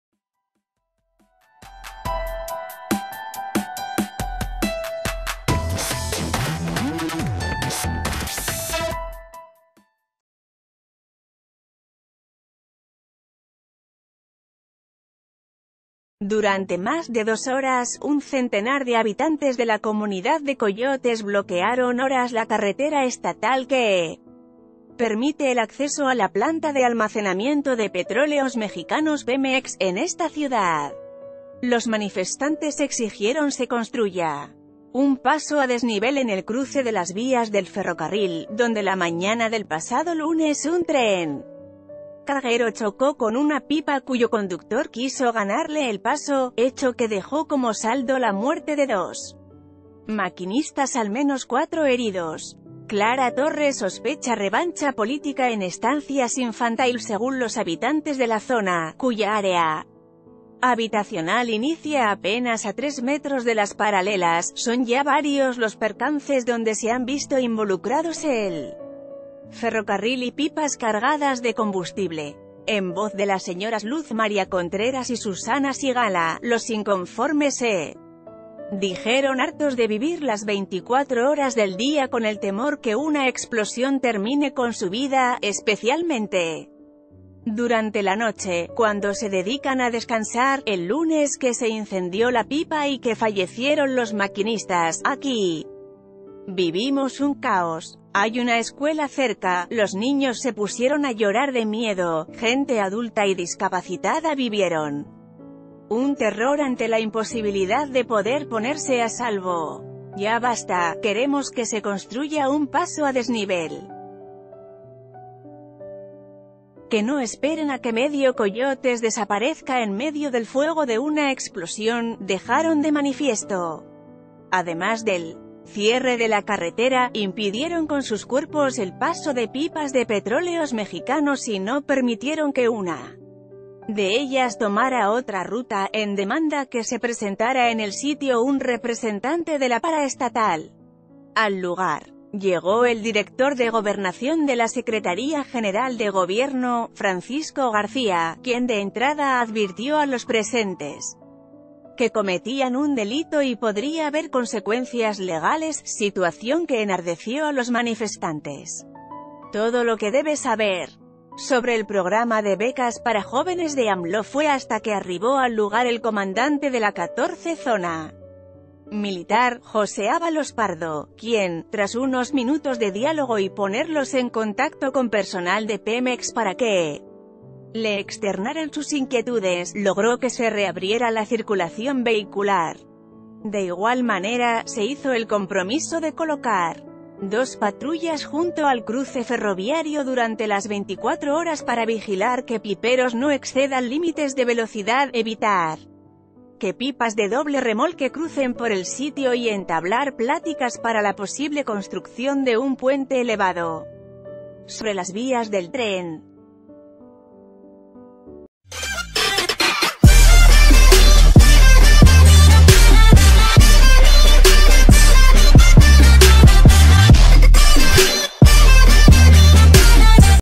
Tras choque de tren con pipa en Aguascalientes, vecinos bloquean carretera
Durante más de dos horas, un centenar de habitantes de la comunidad de Coyotes bloquearon horas la carretera estatal que permite el acceso a la planta de almacenamiento de Petróleos Mexicanos (Pemex), en esta ciudad. Los manifestantes exigieron se construya un paso a desnivel en el cruce de las vías...